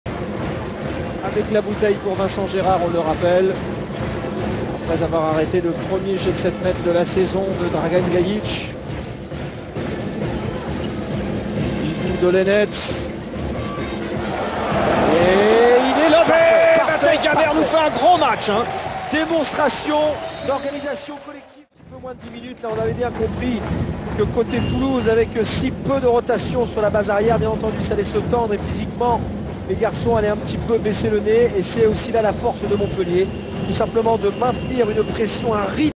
Gol Gaber vs Toulouse
Bonita jugada del Montpellier y gol de Gaber tras pase por detrás de la espalda de Guigou